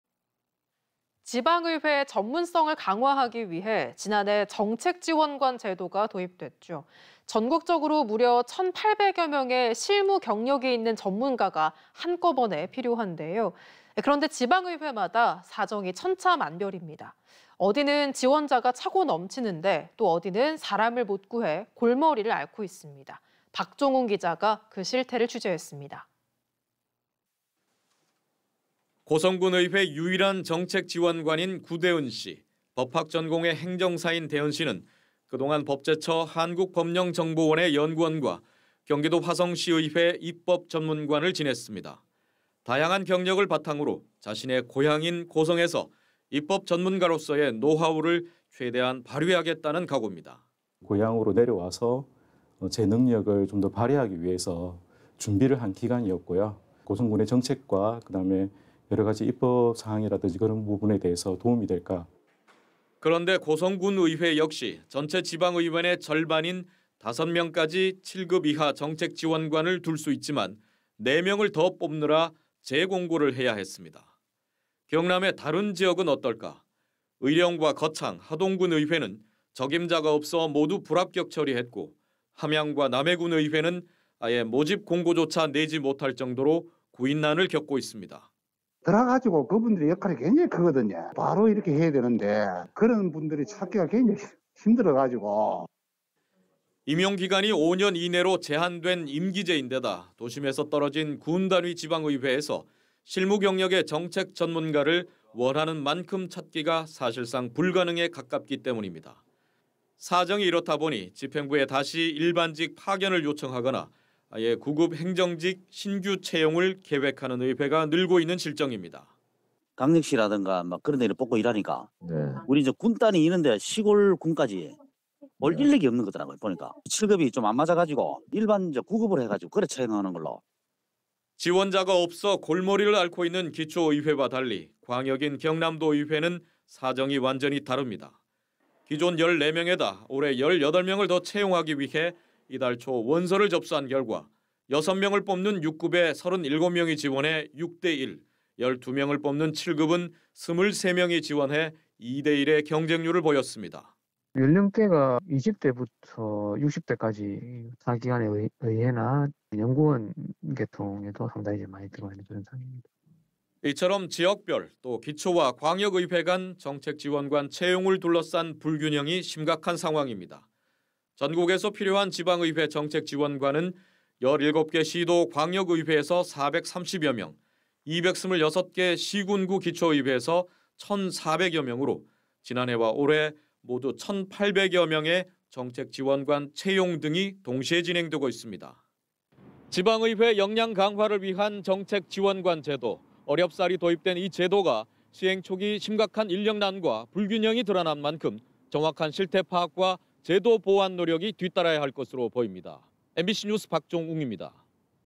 '정책지원관' 군의회는 구인난(2023.1.16/뉴스데스크/MBC경남)
◀ＡＮＣ▶
지방의회 전문성 강화를 위해
지난해 정책지원관 제도가 도입이 됐죠.

전국적으로 무려 천8백여 명의
실무경력이 있는 전문가가 한꺼번에 필요한데
지방의회마다 사정이 천차만별입니다.

어디는 지원자가 차고 넘치는데
어디는 사람을 못 구해 골머리를 앓고 있습니다

박종웅 기자가 그 실태를 취재했습니다.

◀ＥＮＤ▶

◀ＶＣＲ▶
고성군의회 유일한 정책지원관인 구대은 씨.

법학 전공의 행정사인 대은 씨는 그동안
법제처 한국법령정보원의 연구원과
경기도 화성시의회 입법전문관을 지냈습니다.

다양한 경력을 바탕으로 자신의 고향인
고성에서 입법전문가로서의 노하우를
최대한 발휘하겠다는 각옵니다.

◀ＩＮＴ▶구대은 / 고성군의회 정책지원관
"고향으로 내려와서 제 능력을 좀 더 발휘하기 위해서 준비를 한 기간이었고요. 고성군의
정책과 여러가지 입법사항이라든지
그런 부분에 대해서 도움이 될까"

그런데 고성군의회 역시 전체 지방의원의
절반인 5명까지 7급이하 정책지원관을
둘 수 있지만 4명을 더 뽑느라
재공고를 해야 했습니다.

경남의 다른 지역은 어떨까?

의령과 거창, 하동군의회는
적임자가 없어 모두 불합격 처리했고
함양과 남해군의회는
아예 모집 공고조차 내지 못할 정도로
구인난이 심각합니다.

◀ＳＹＮ▶임태식 / 남해군의회 의장
"들어와서 그 분(정책지원관)들의 역할이 굉장
히 크거든요. 바로 (업무를)해야 되는데 그런
분들을 찾기가 굉장히 힘들어가지고..."

임용기간이 5년 이내로 제한된 임기제인데다
도심에서 떨어진 군단위 지방의회에서
실무경력의 정책전문가를 원하는 만큼 찾기가
사실상 불가능에 가깝기 때문입니다

사정이 이렇다보니
집행부에 다시 일반직 파견을 요청하거나
아예 9급 행정직 신규 채용을 계획하는 의회가 늘고 있는 실정입니다.

◀ＳＹＮ▶진학성 / 거창군의회 의사담당
"광역시 라든가 그런 데서 뽑으니까
군단위 이런 데 시골군까지 올 인력이 없는 것
같더라고요. (임기제)7급이 좀 안맞아가지고
일반 9급으로 채용하는 걸로..."

지원자가 없어 골머리를 앓고 있는
기초의회와 달리
광역인 경남도의회의는 사정이 완전히 다릅니다

(CG)기존 14명에다 올해 18명을 더 채용하기
위해 이달 초 원서를 접수한 결과
6명을 뽑는 6급에 37명이 지원해 6대 1,
12명을 뽑는 7급은 23명이 지원해
2대 1의 경쟁률을 보였습니다.

◀ＳＹＮ▶류원/경남도의회 인사교육담당사무관
"(지원자)연령대가 20대부터 60대까지,
타기관의 의회나 관련 연구원 계통도
상당히 많이 들어와 있는 상황입니다"

이처럼 지역별, 또 기초와 광역의회간
정책지원관 채용을 둘러싼 불균형이
심각한 상황입니다.

전국에서 필요한 지방의회 정책지원관은
17개 시도 광역의회에서 430여 명,
226개 시군구 기초의회에서 천4백여 명으로
지난해와 올해 모두 천8백여 명의
정책지원관 채용 등이 동시에 진행되고
있습니다.

(S/U)
지방의회 역량 강화를 위한 정책지원관제도,
어렵사리 도입된 이 제도가
시행초기 심각한 인력난과 불균형이 드러난만큼
정확한 실태파악과 제도보완 노력이
뒤따라야 할 것으로 보입니다.
MBC NEWS 박종웅 ◀ＥＮＤ▶

◀리포트 직후 ◀ＡＮＣ▶
내일 이시간에는 정책지원관 제도 안착을 위한
대안은 없는지 짚어보겠습니다.
◀ＥＮＤ▶